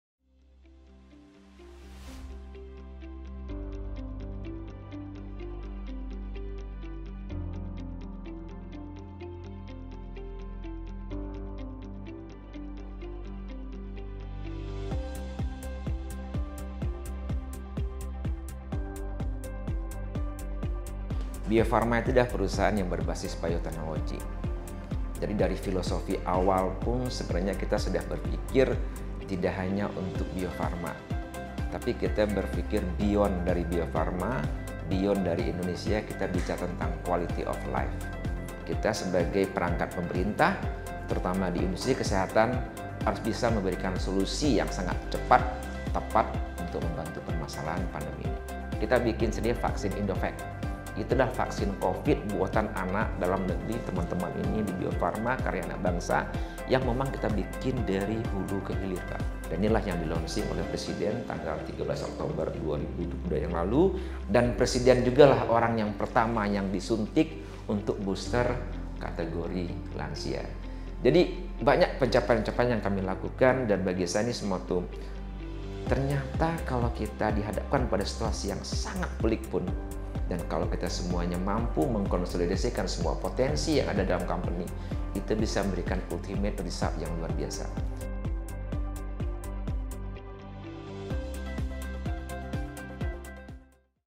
Cuplikan Presentasi Direktur PT BIO FARMA(PERSERO)
[PRESENTASI CEO PT BIO FARMA (PERSERO)]

Halo SobatHijau🍃

Yuk, kita dengarkan cuplikan singkat Bapak Honesti Basyir, Direktur Utama PT Bio Farma.

SobatHijau, yuk simak video berikut ini ya.